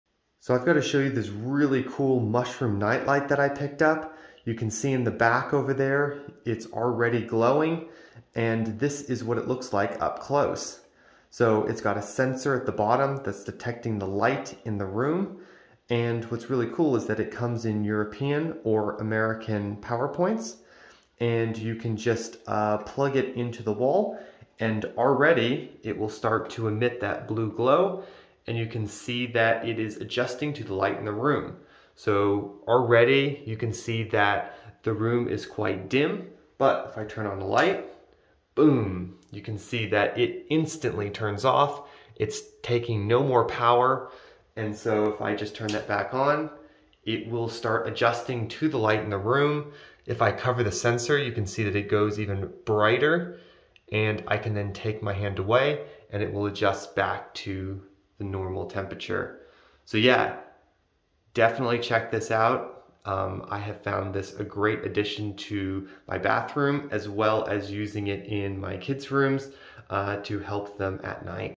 What Makes the Mushroom Night Light the Perfect Addition for Your Bathroom?
Illuminate Your Rooms with the Mushroom Night Light!

Enhance your surroundings with the Mushroom Night Light. Its gentle glow creates a cozy ambiance, perfect for relaxation or bedtime. Say goodbye to harsh lighting and hello to soothing illumination with this adorable mushroom-shaped night light.